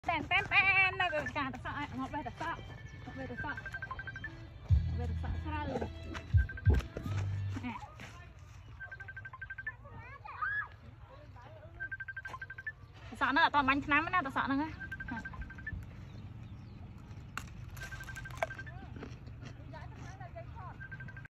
Mastering the Art of Cucumber Picking
Get ready for a crunchy adventure in cucumber picking! Dive into the world of vine-grown delights as we explore the ins and outs of harvesting these refreshing gems. From choosing the best cucumbers to picking techniques and preserving freshness, this video is your ultimate guide to cucumber perfection. Whether you're a garden enthusiast or simply love farm-fresh produce, join us as we uncover the secrets to picking cucumbers at their peak @Nyka1